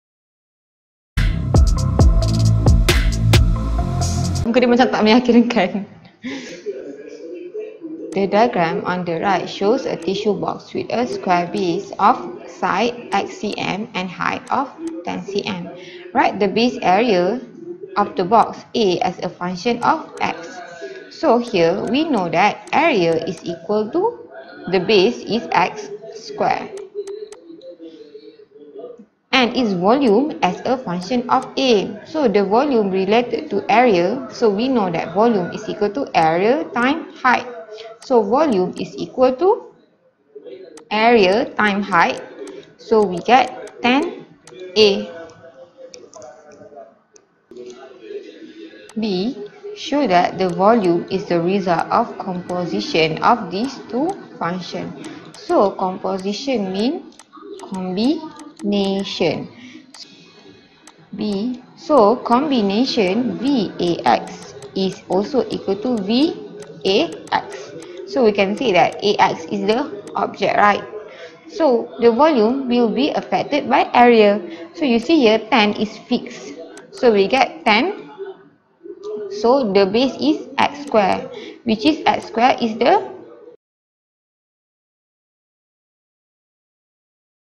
Functions intensive practice 1.2 Q6 latihan intensif 1.2 add maths form 4 textbook kssm
Functions intensive practice 1.2 Q6 latihan intensif 1.2 add maths form 4 textbook buku teks matematik tambahan kssm

The diagram on the right shows a tissue box with a square base of
side x cm and the height of 10 cm.
(a) Write the base area of the box, A as a function of x and its
volume, V as a function of A.
(b) Show that the volume, V is the result of the composition of
these two functions.

composite functions